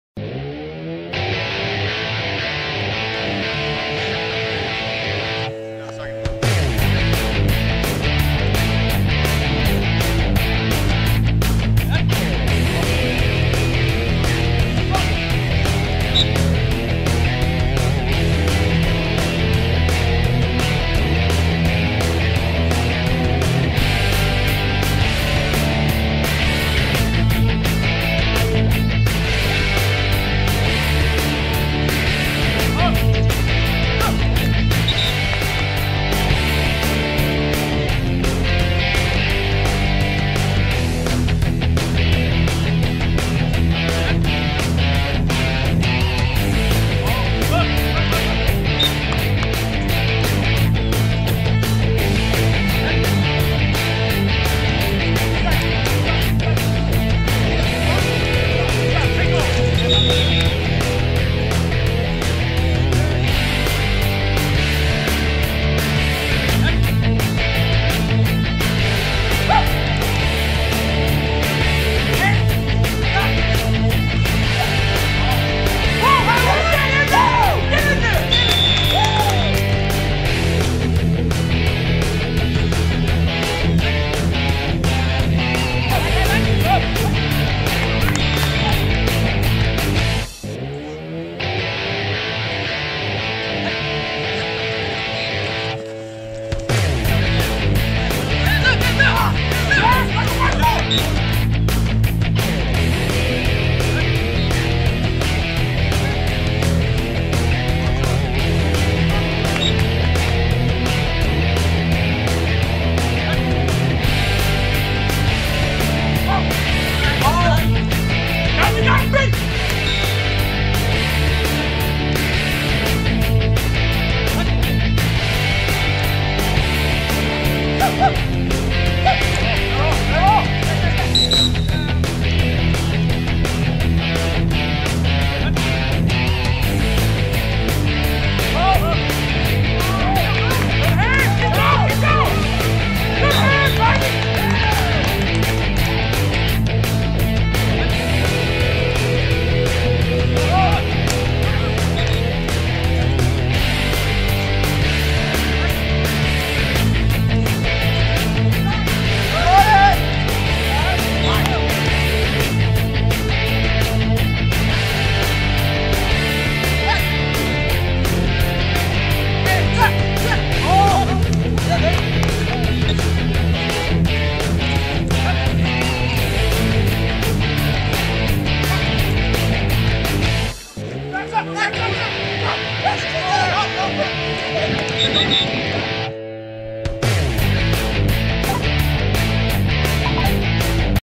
MUSC CDM 2015 Intramural Football
Random compilation of offensive plays from our game against the CDM 2017... We won